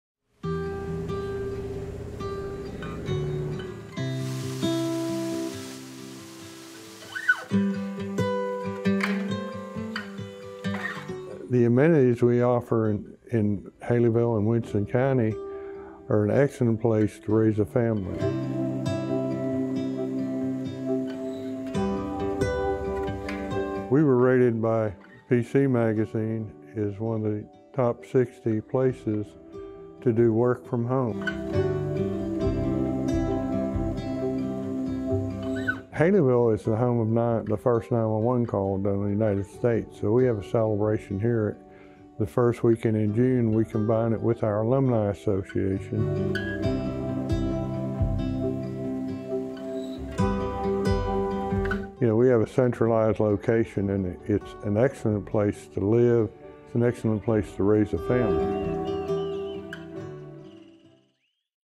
Retire to Winston County
Retire to beautiful Winston County Alabama. Home to Smith Lake and conveniently located between Birmingham and Huntsville.